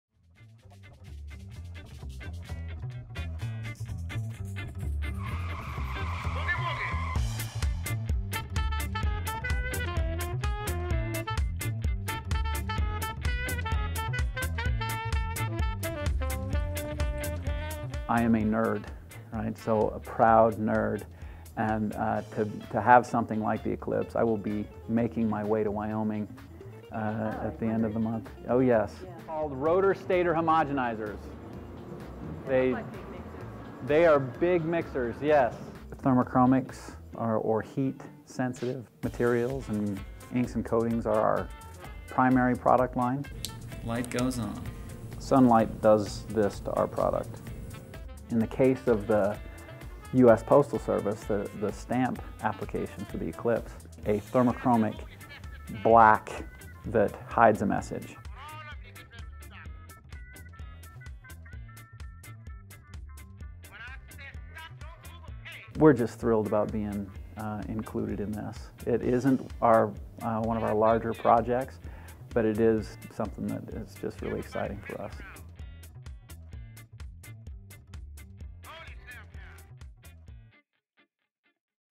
Solar eclipse memorialized with heat-activated stamp, ink made in Colorado Springs
The US Postal Office issued a special stamp commemorating the solar eclipse, the first stamp made with heat-activated ink to reveal the moon under the shadow. Chromatic Technologies Inc. developed the ink used in the stamp, and founder Lyle Small says the innovative technology has diverse applications beyond labels and beer cans.

Video by Maggie Andresen, The Denver Post